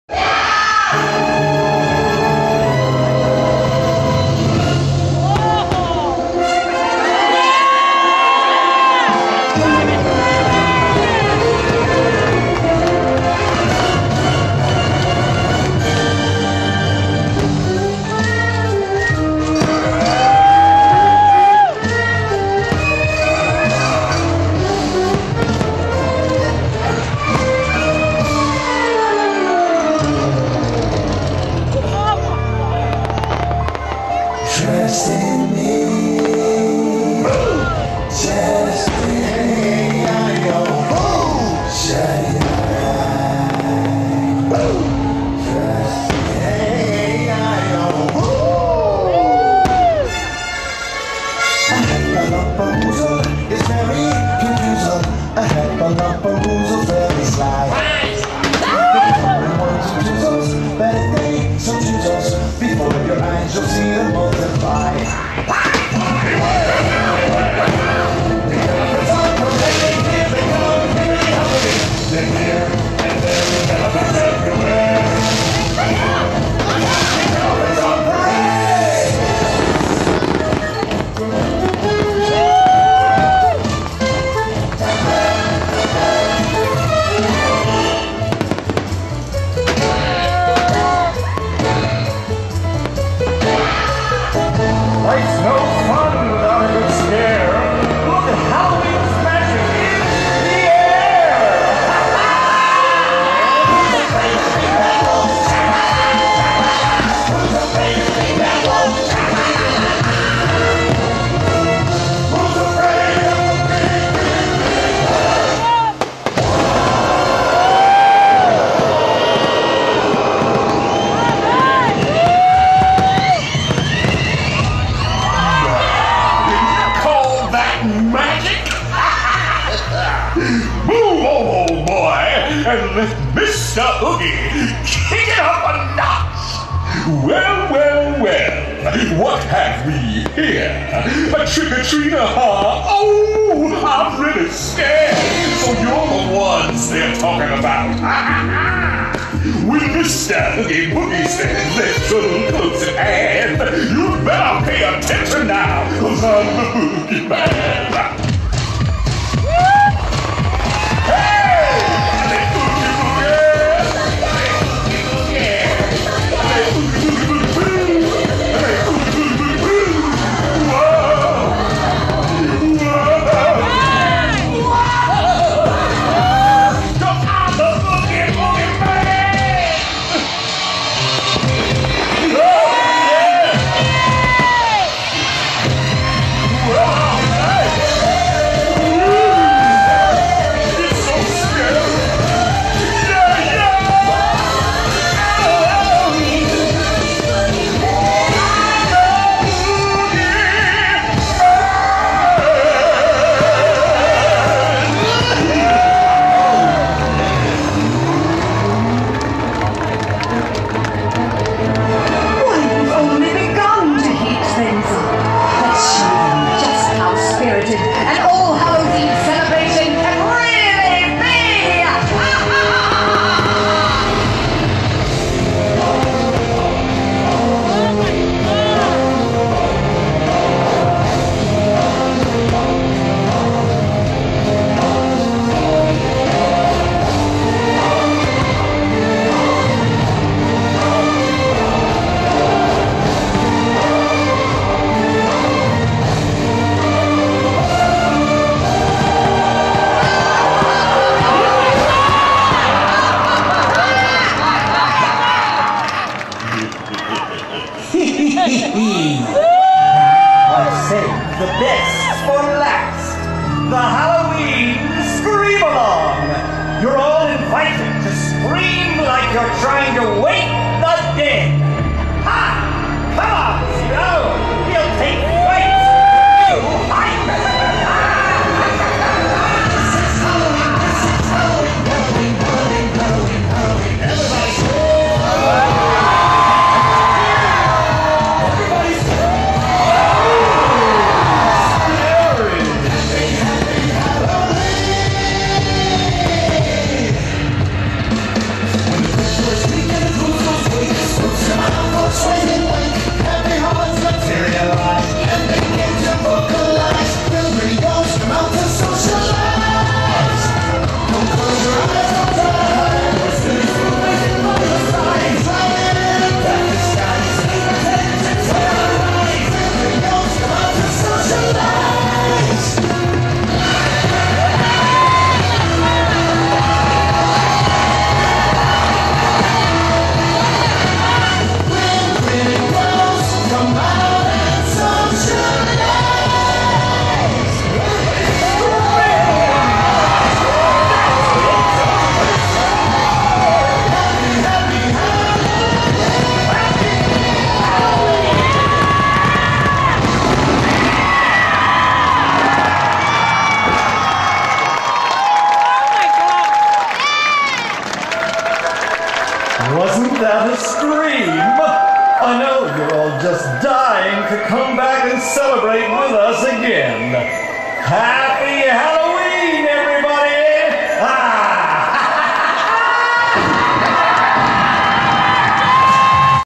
Halloween Screams - Part 2
The public debut of Disneyland's new Halloween-themed fireworks, Halloween Screams. Part 2 of 2. Presented by MousePlanet.com.